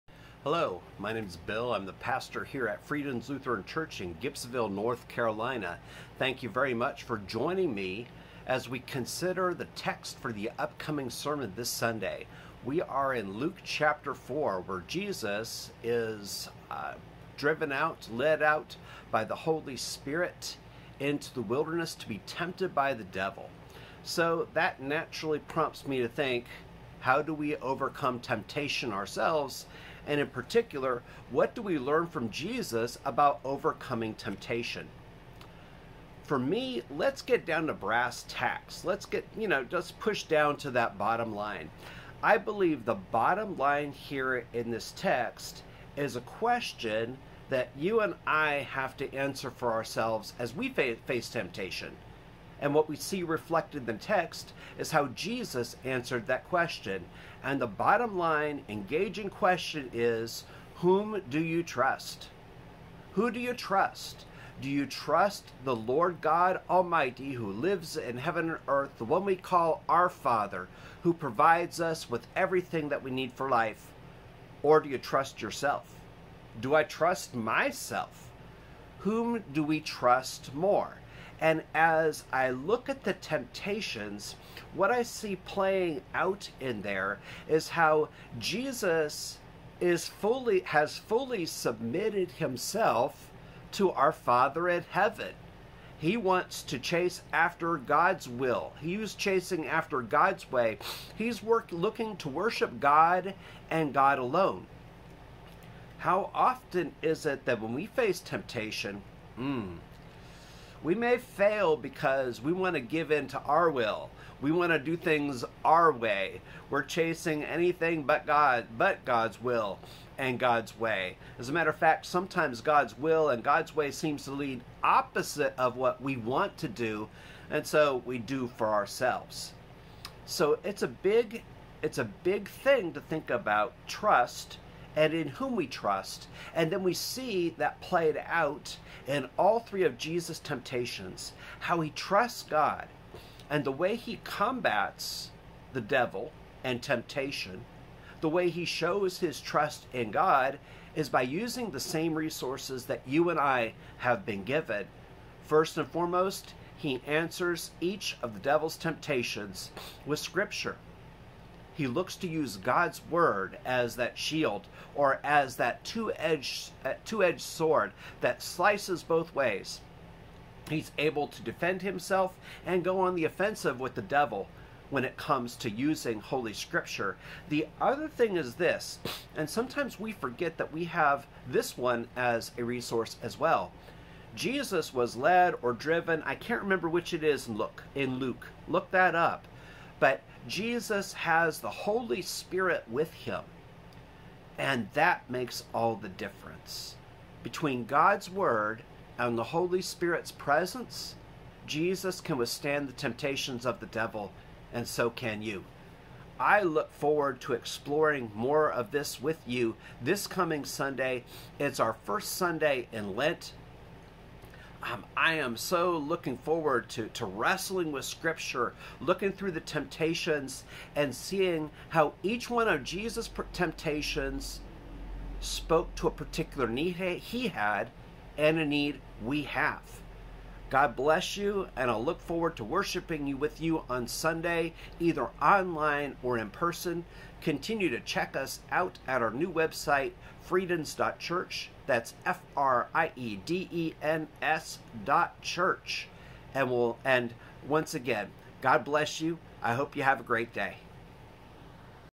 Sermon Study Mar.6...Trust Overcomes Temptation
Jesus teaches us to overcome temptation with two things.